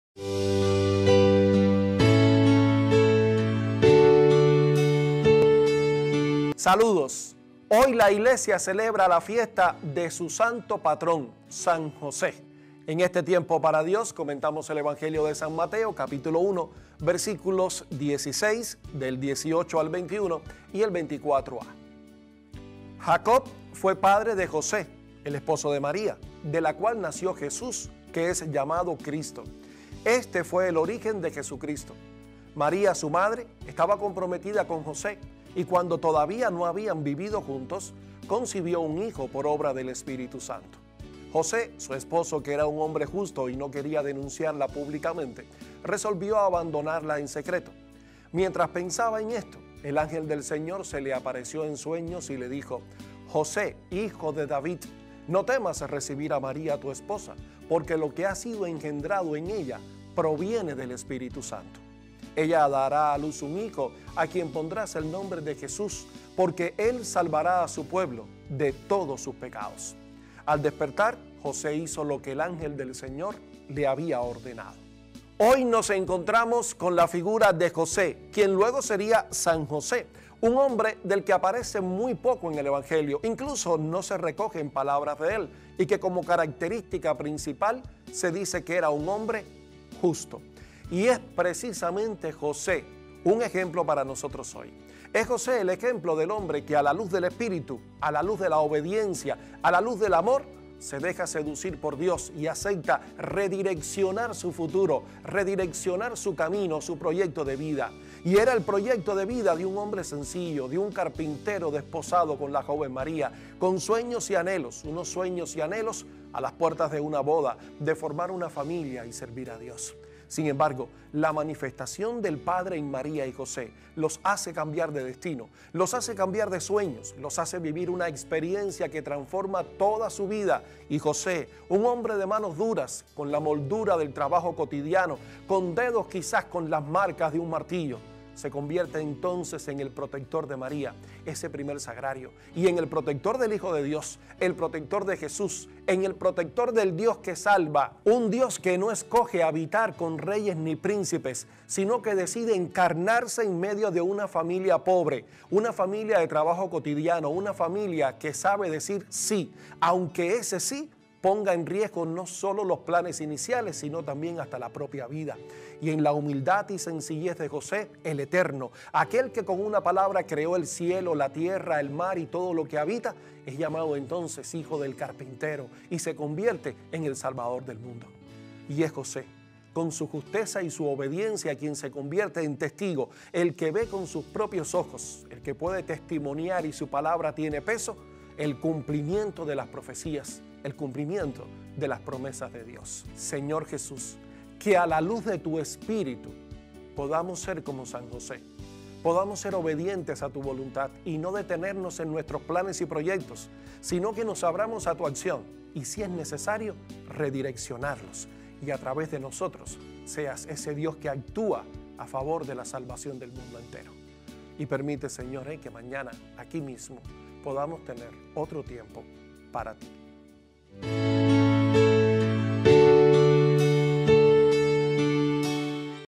EVANGELIO DE HOY VIERNES 19 DE MARZO DE 2021. San Mateo 1,16.18-21.24a
Es José el ejemplo del hombre que a la luz del Espíritu, a la luz de la obediencia, a la luz del amor, se deja seducir por Dios y acepta re-direccionar su futuro, re-direccionar su camino, su proyecto de vida.